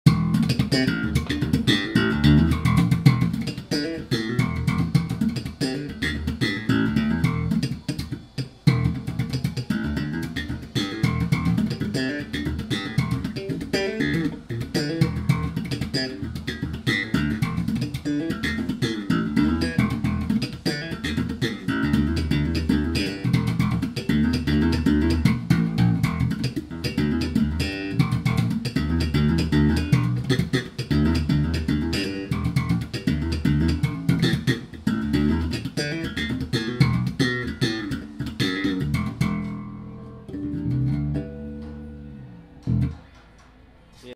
Ken Smith BHF5-BW ユーザーレポート！ （Slap Improvisation）
こんにちは！島村楽器神戸三宮店ベース担当の川口です！今回はロイヤルカスタマーユーザーレポート！Vol.1!!ということでSchloderのベーシスト・タツヤさんに出演してもらいました！スラップなんか弾いてください！とお願いしたら快く弾いてくれました！関西ベース情報発信ブログ BASE OF BASS 川口の出勤日に更新中！応援宜しくお願い致します！